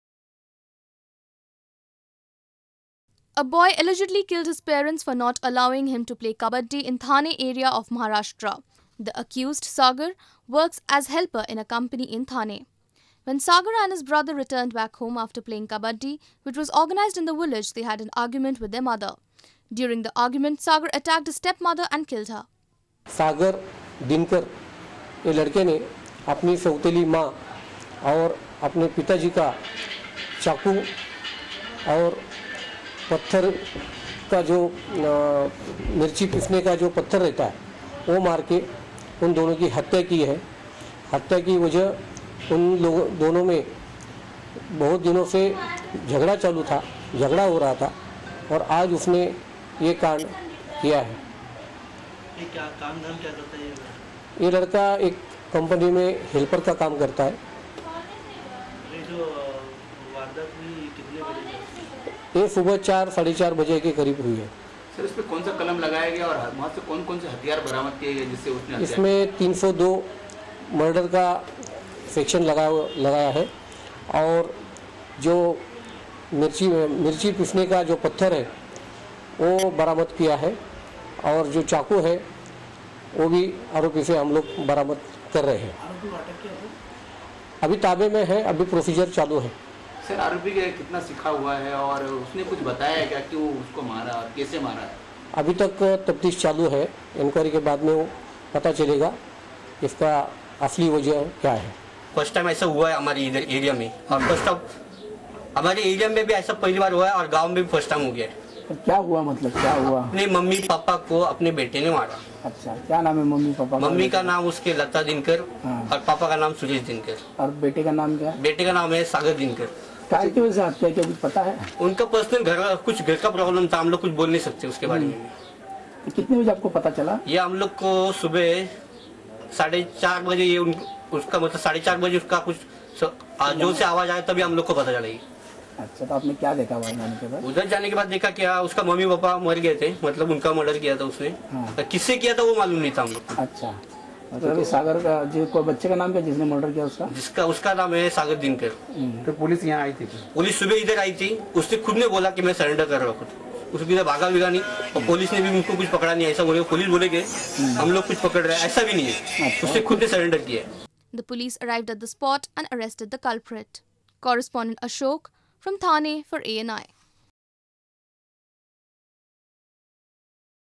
Thane’s boy kills parents over Kabaddi dispute
Thane, Mar 08 (ANI): A boy allegedly killed his parents for not allowing him to play Kabbadi in Thane area of Maharashtra. The accused, Sagar, works as helper in a company in Thane. When Sagar and his brother returned back home after playing Kabbaddi, which was organised in the village, they had an argument with their mother. During the argument, Sagar attacked his step mother and killed her. The police arrived at the spot and arrested the culprit.